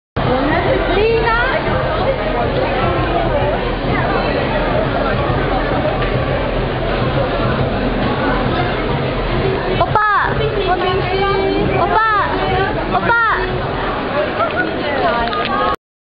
Won Bin Burberry Singapore 031109 Part 2
Won Bin Oppa About To Leave the Burberry Event 031109